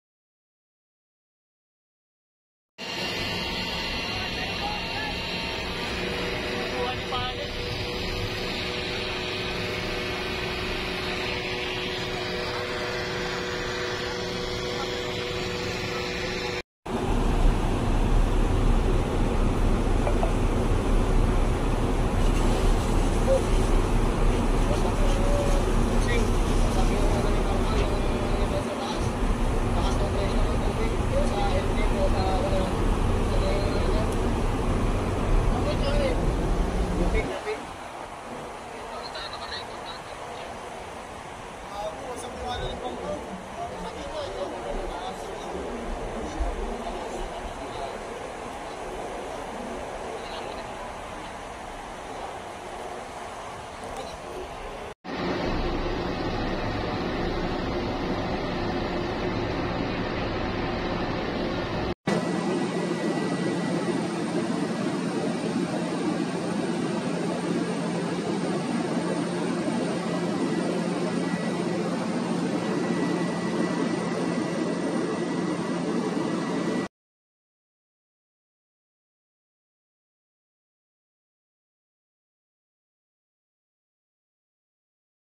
China Coast Guard fires water cannon at BFAR vessels in Bajo de Masinloc
A China Coast Guard (CCG) ship blasted water cannon at Bureau of Fisheries and Aquatic Resources (BFAR) vessels that were delivering supplies to fisherfolk communities in Bajo de Masinloc (Scarborough/Panatag Shoal) on Saturday, Dec. 9, the latest in a string of aggressive incidents shown by Chinese vessels within the Philippine waters. (Video Courtesy of Philippine Coast Guard/AFP)